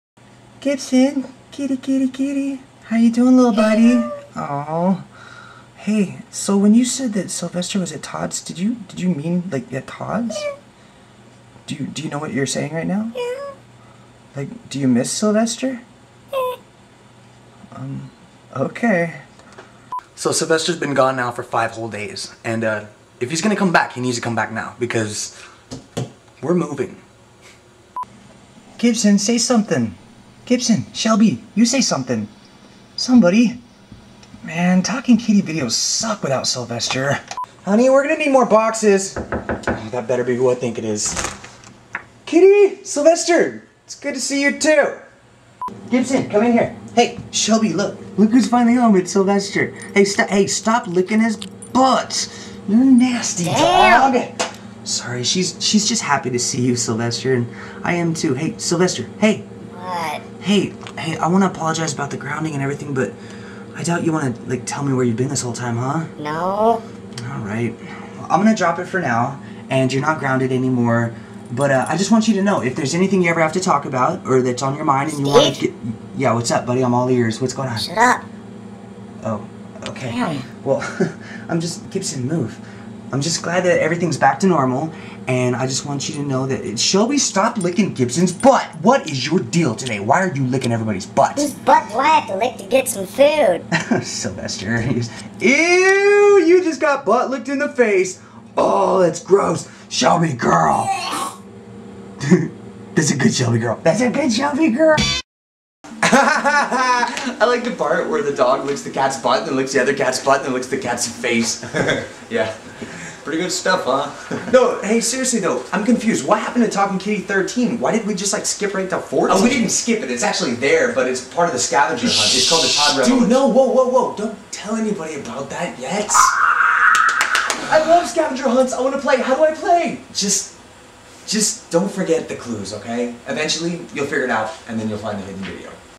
Talking Kitty Cat 14 - Butt Licked In The Face
Sylvester the talking kitty cat gets a nasty welcome home after a five day outing.  Also in this episode: Gibson makes verbal progress and Shelby licks some butts.